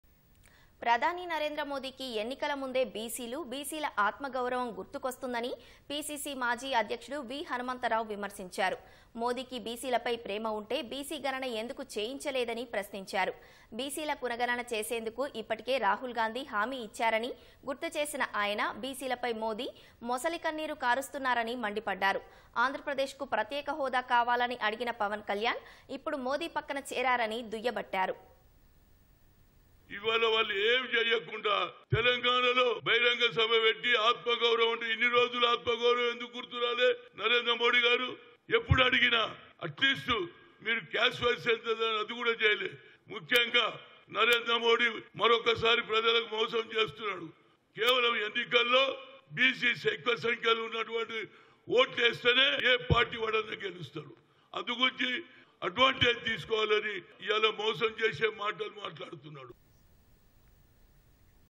V.Hanumanta Rao Satires on PM Modi -Pawan Kalyan | Demands For Caste Cenus | మోదీవి మొసలి కన్నీళ్లు!
ప్రధాని నరేంద్ర మోదీకి ఎన్నికల ముందే B.Cలు, B.Cల ఆత్మగౌరవం గుర్తుకొస్తుందని P.C.C మాజీ అధ్యక్షుడు వి.హనుమంతరావు విమర్శించారు. మోదీకి B.Cలపై ప్రేమ ఉంటే, B.C గణన ఎందుకు చేయించలేదని ప్రశ్నించారు.

B.Cలపై మోదీ...మొసలి కన్నీరు కారుస్తున్నారని మండిపడ్డారు. ఆంధ్రప్రదేశ్‌కు ప్రత్యేక హోదా కావాలని అడిగిన పవన్ కళ్యాణ్ ఇప్పుడు మోదీ పక్కన చేరారని దుయ్యబట్టారు.